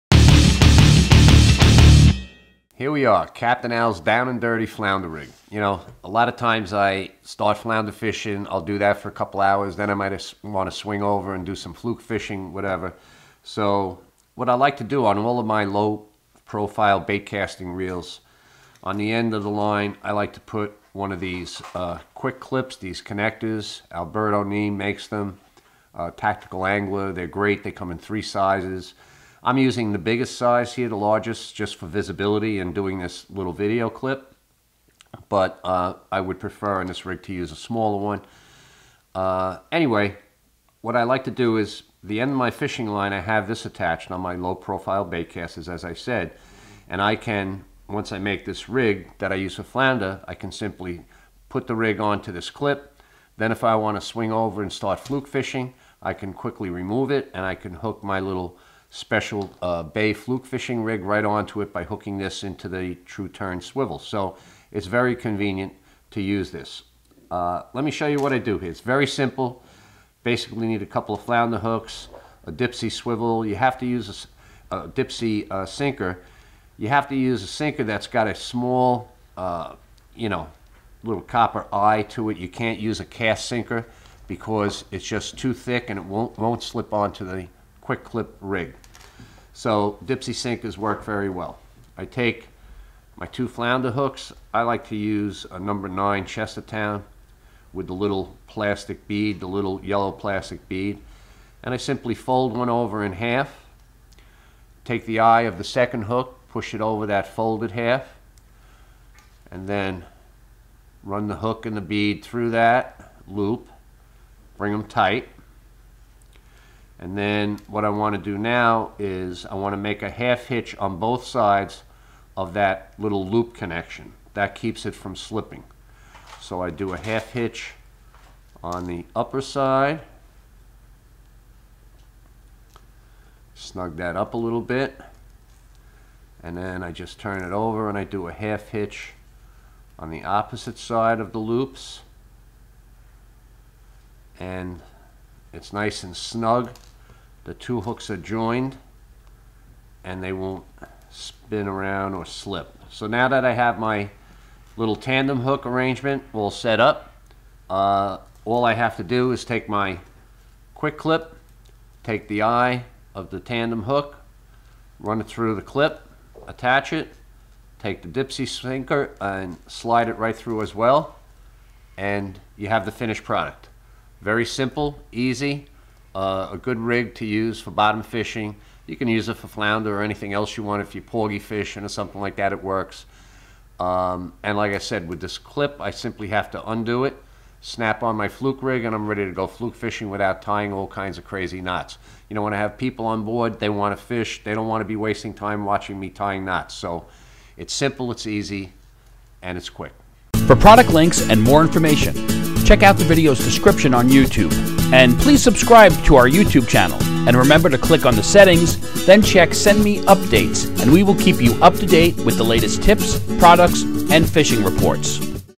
Captain Al Lorenzetti's Quick Winter Flounder Rig
Learn how to tie Captain Al Lorenzetti's quick winter flounder rig.